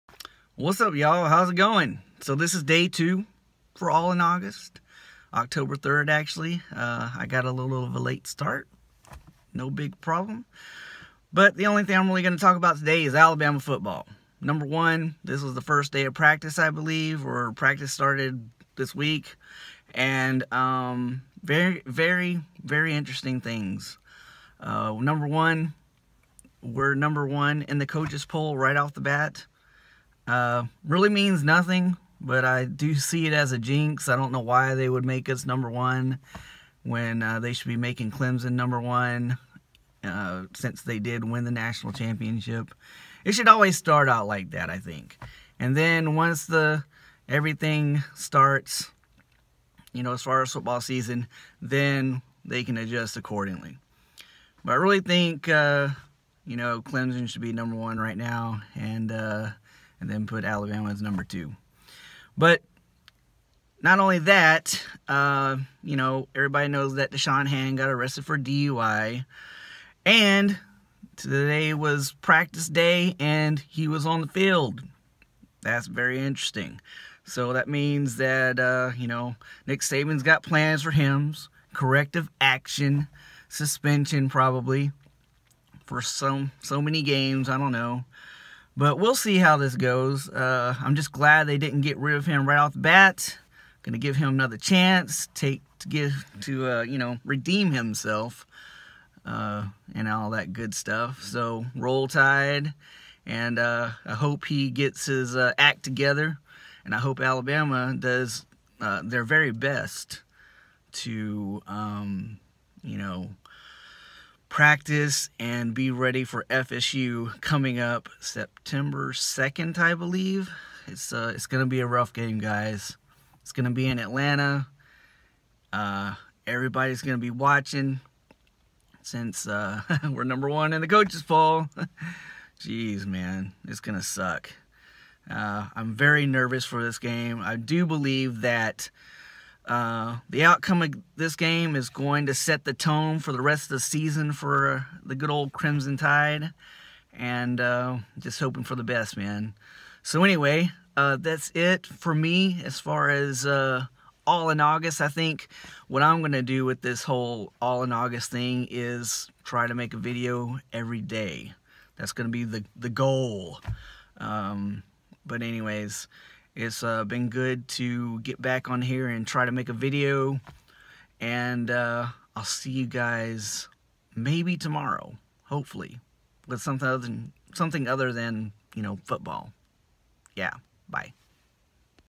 Alabama Football - Da'Shawn Hand Practicing After Being Arrested For DUI | AllinAugust Day #2
Da'Shawn Hand Practicing After Being Arrested For DUI | AllinAugust Day #2

The channel down below is the inspiration for this video.  Heather is a pretty good vlogger among other things.  Check it out.

More links to come when I get everything updated.